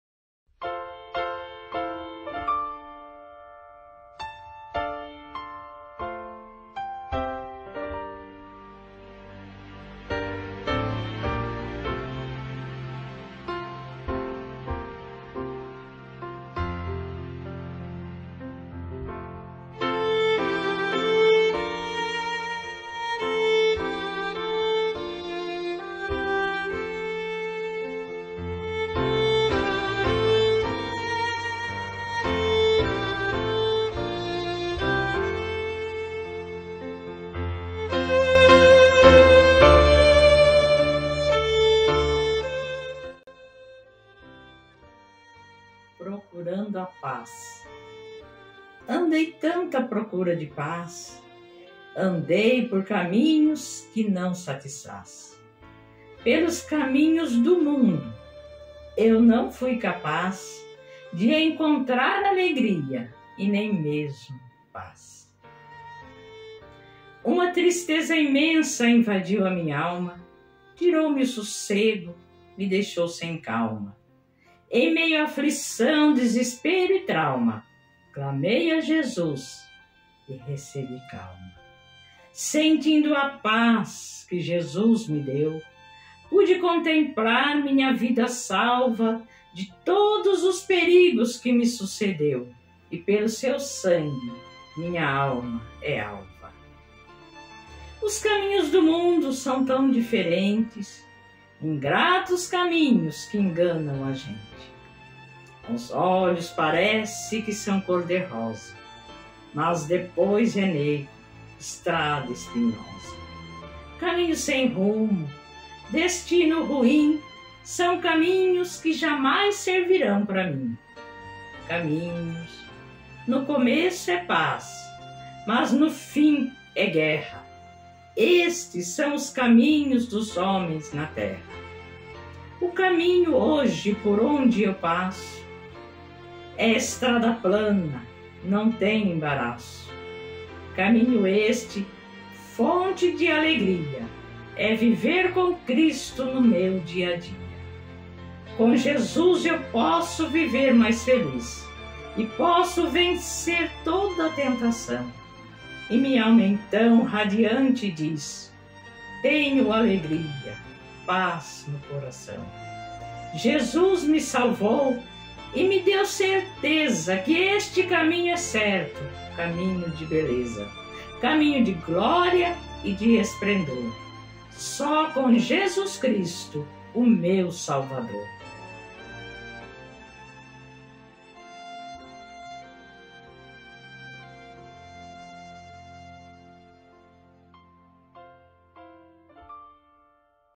PROCURANDO A PAZ !
Escrito e interpretado por Maria Aparecida Lima.
Um poema que vai tocar bem fundo na sua alma, e te mostra onde encontrar a verdadeira paz.